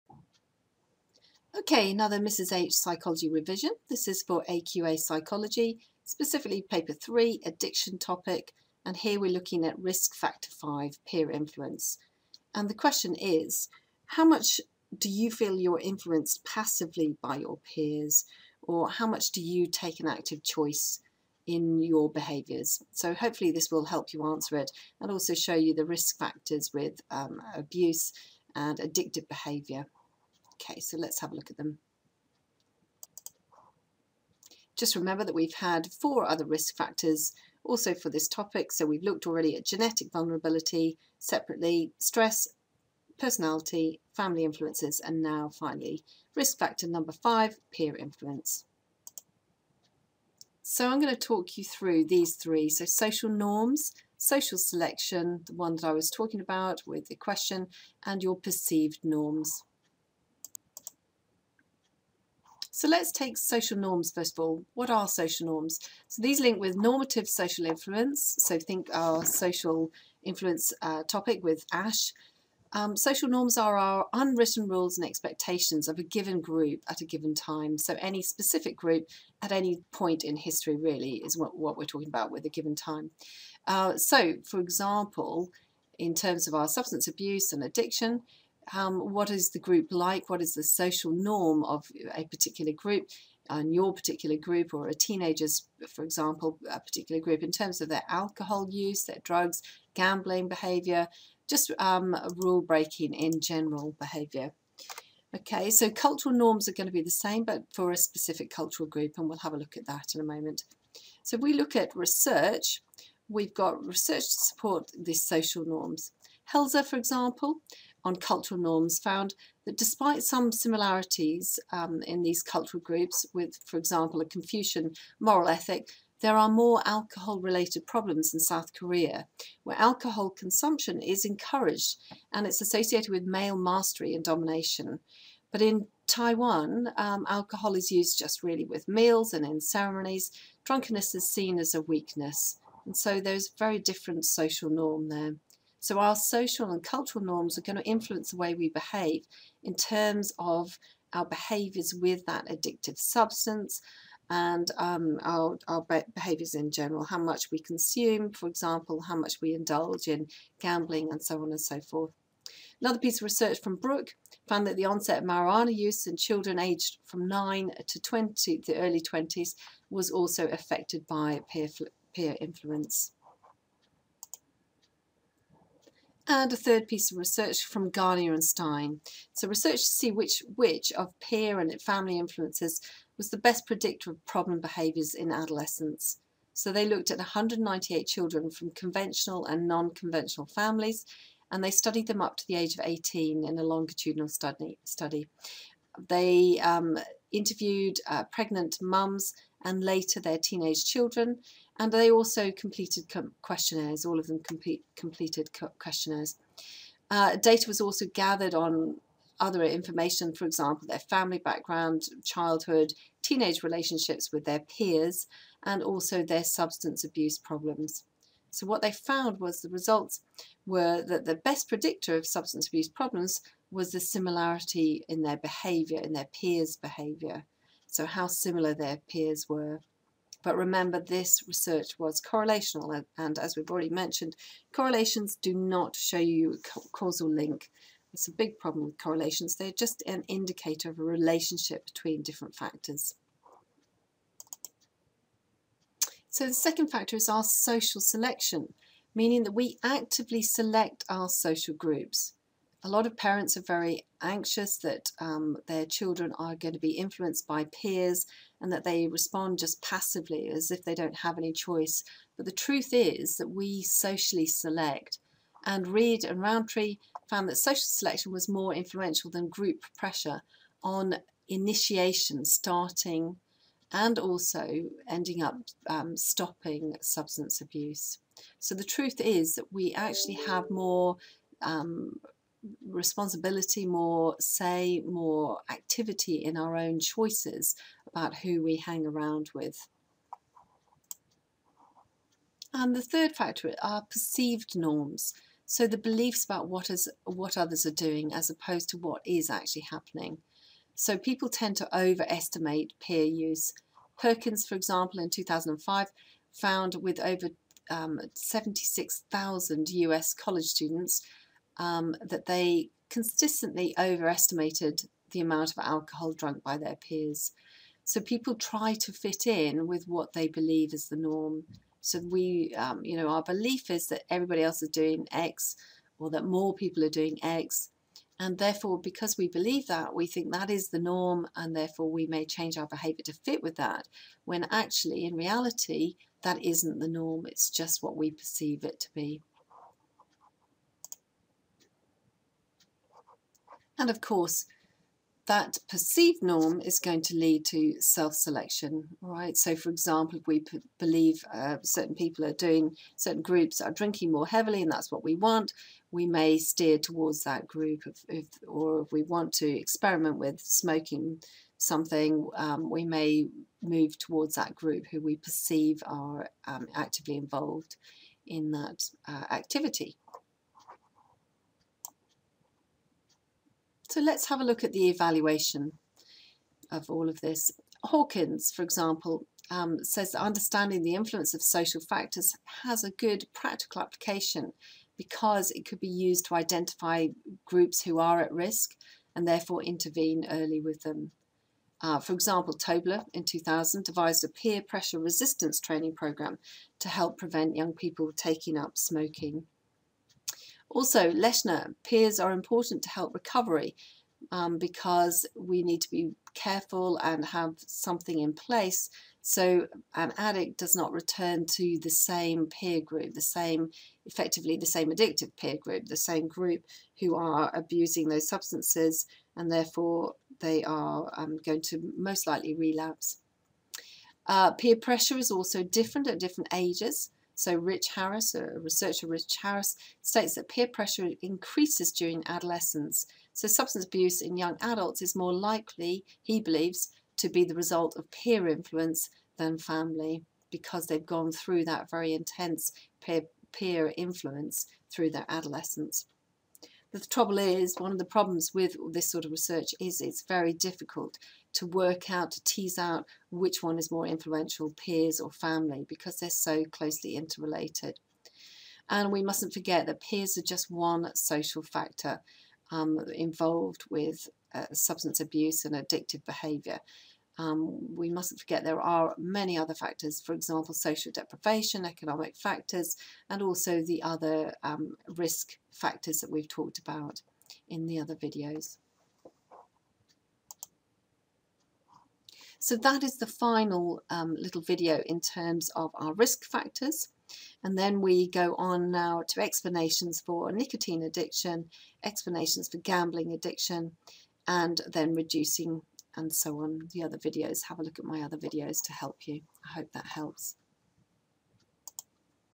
Addiction 2 A Level AQA: risk factor 5: peers
AQA Psychology: Addiction topic: video 2 risk factor 5 in Addiction, Peer influence